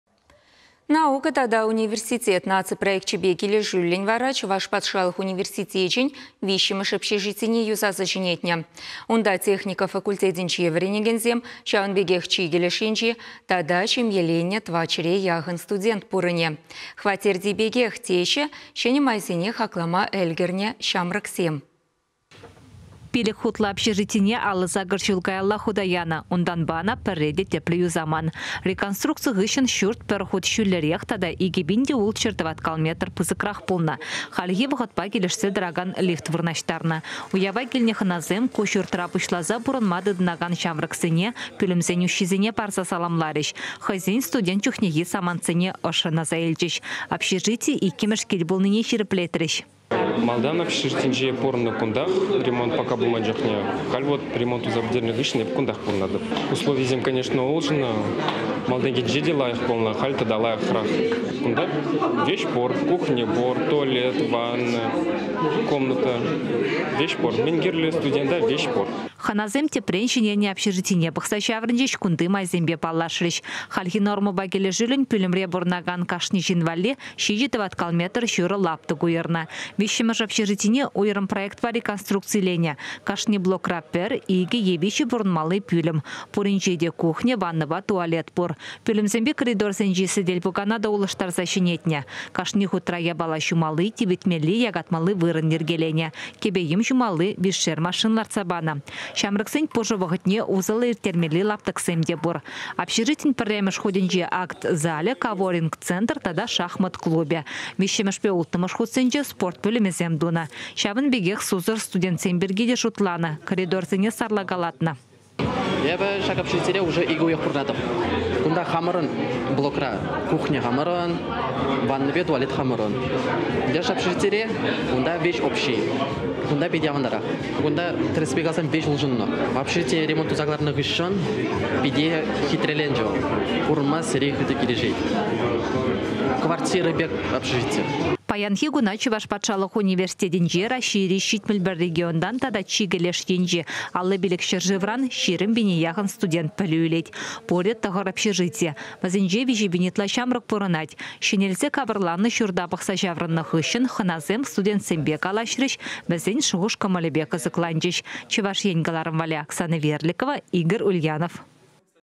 Наци проекчӗпе килӗшӳллӗн Чӑваш патшалӑх университечӗн 3-мӗш общежитине юсаса ҫӗнетнӗ
Унта техника факультетӗнче вӗренекенсем, ҫавӑн пекех чикӗ леш енчи тата ҫемьеленнӗ 400 яхӑн студент пурӑнӗ. "Хваттерти пекех" теҫҫӗ, ҫӗнӗ майсене хаклама ӗлкӗрнӗ ҫамрӑксем.